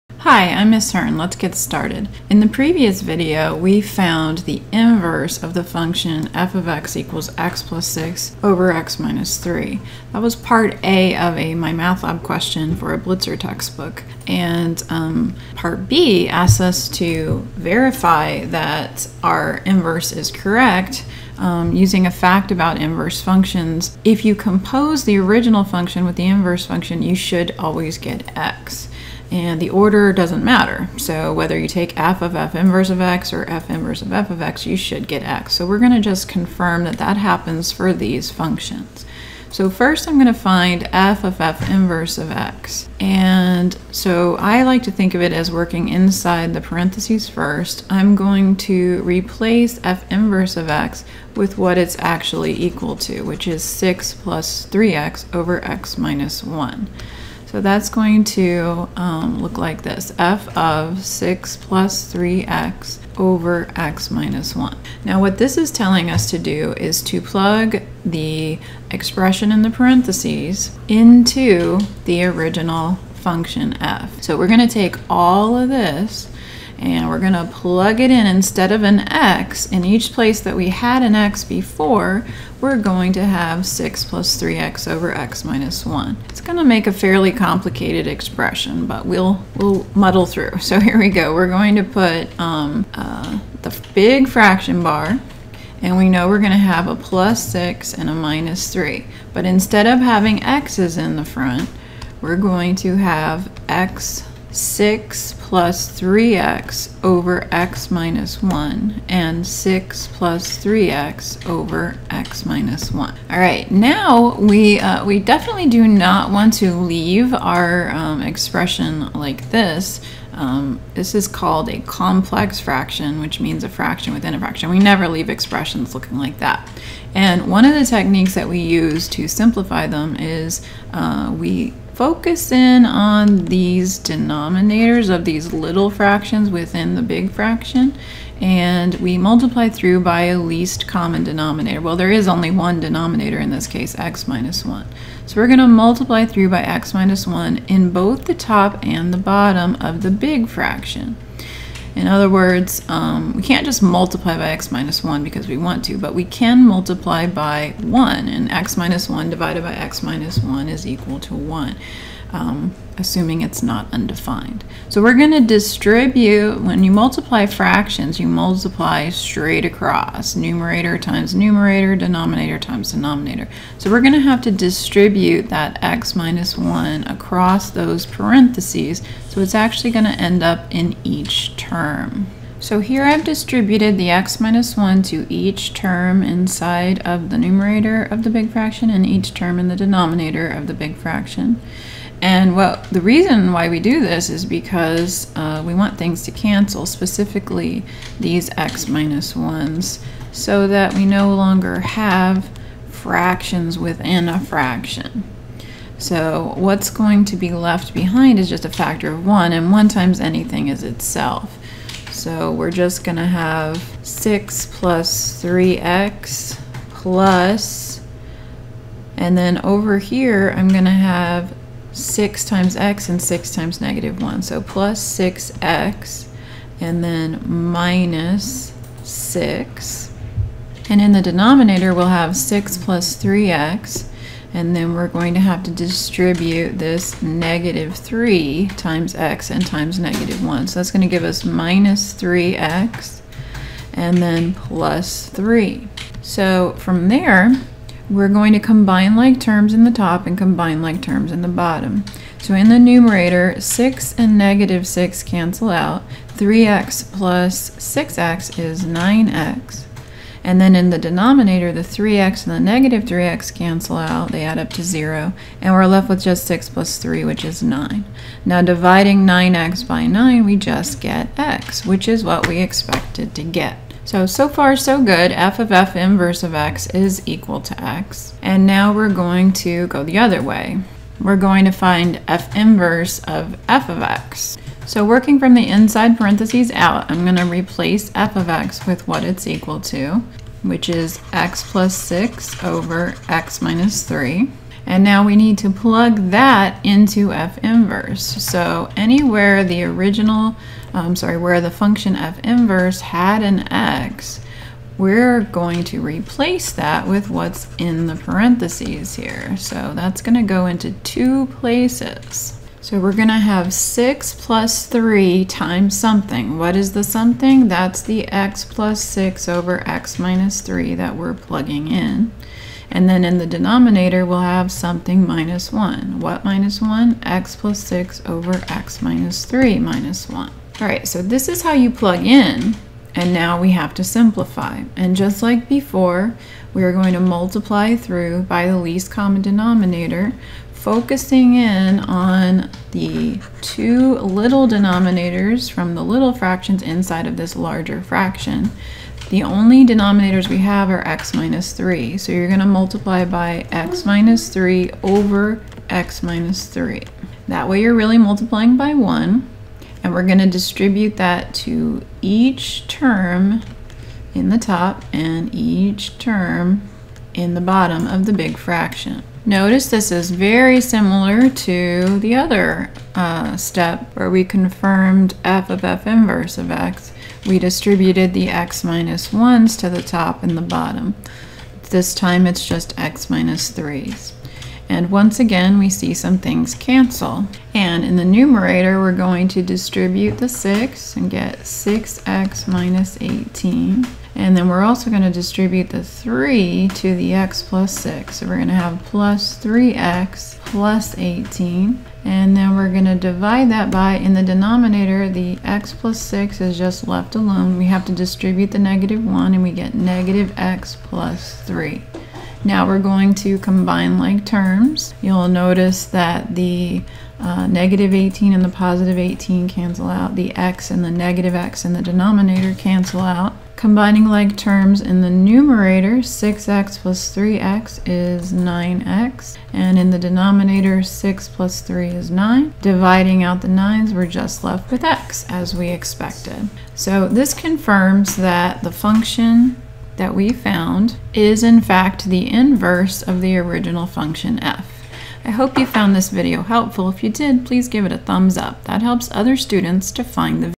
Inverse Functions Part 7: Confirm a Pair of Rational Functions are Inverses
In Part 5, we found the inverse of the function f(x)=(x+6)/(x-3).  In this video, we confirm that the function y=(6+3x)/(x-1) is the inverse of the function f(x)=(x+6)/(x-3).